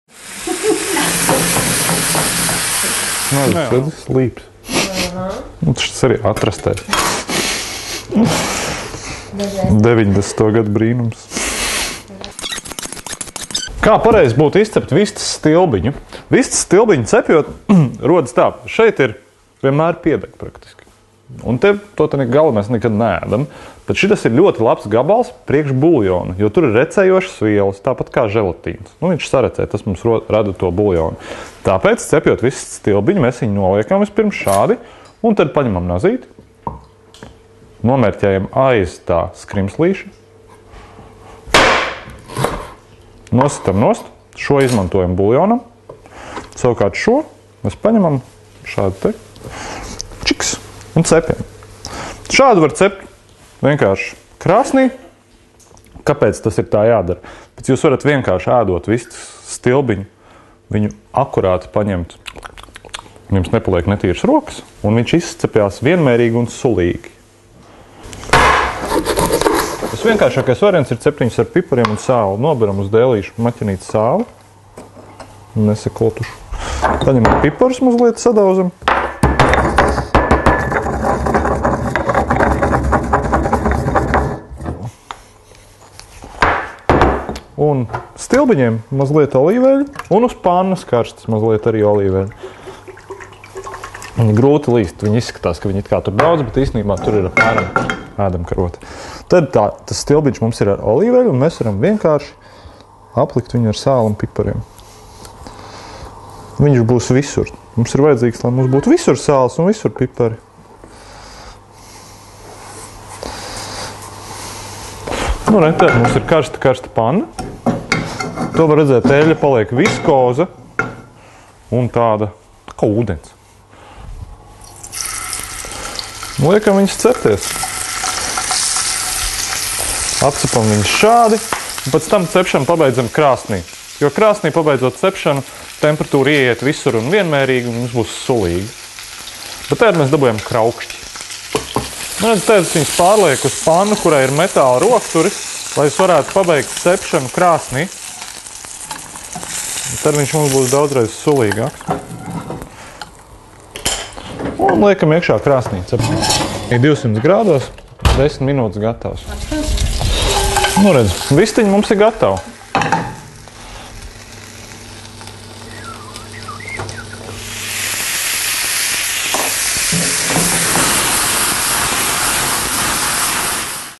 Vistu stilbiņu cepšana
Pavāra Mārtiņa Sirmā padoms, kā vislabāk sagatavot vistu kājiņas cepšanai + 1 pavisam vienkārša recepte, kuras iznākums ļooti gards!
Ģimenes portāls mammam.lv/tetiem.lv piedāvā virtuves gudrības un knifiņus kopā ar pavāru Mārtiņu Sirmo.